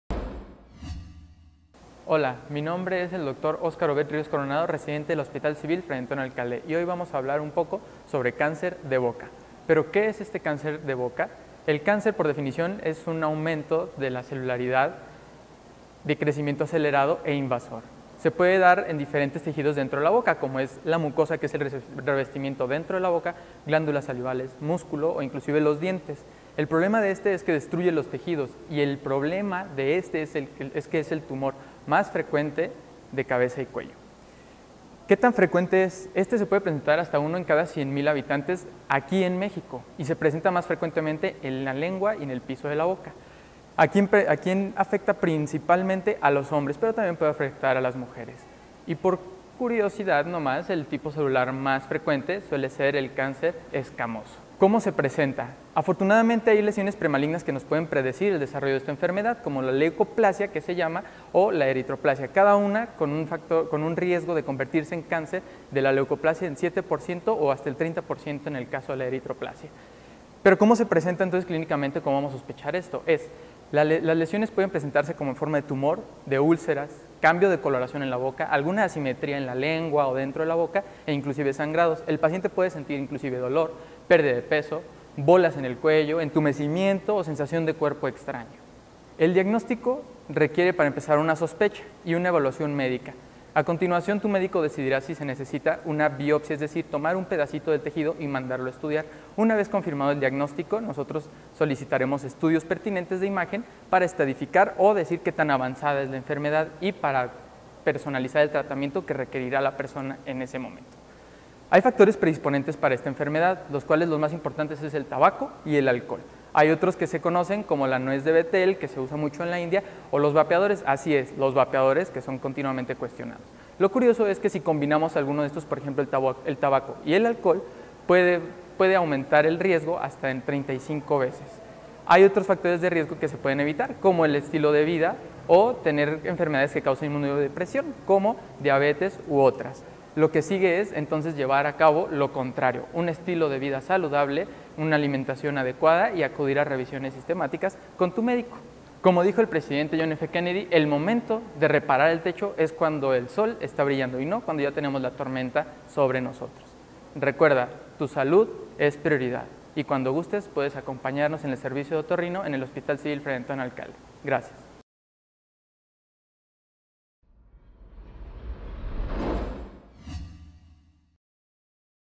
Cáncer de boca | Especialistas HCG
¿Qué es el cáncer de boca? Explicación de un experto de Hospital Civil de Guadalajara. Infórmate más sobre el cáncer de boca, también llamado “cáncer bucal”, con el Dr. Oscar Obeth Ríos Coronado